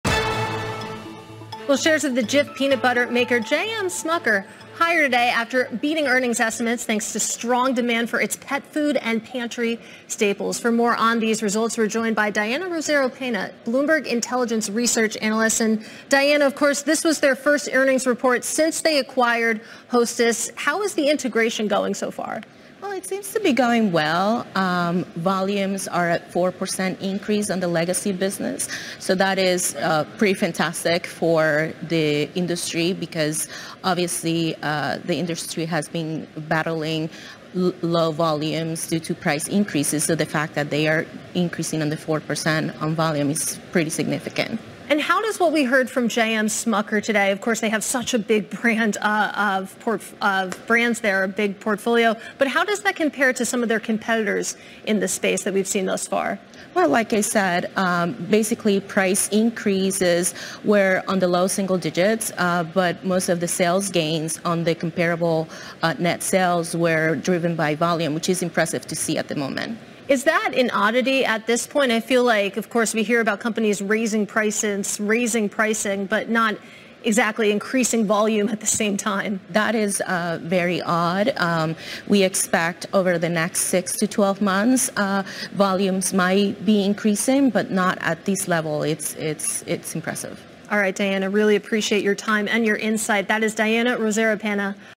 J M Smucker 3Q Adjusted EPS Beats Estimates (Feb. 27, 2024)
Consumer Staples Equity Analyst Diana Rosero-Pena provides a jam-packed analysis of J M Smucker's earnings, evaluates the smoothness of their recent integration, and compares their position with competitors.

For more data and research visit { BI } on your Bloomberg Terminal.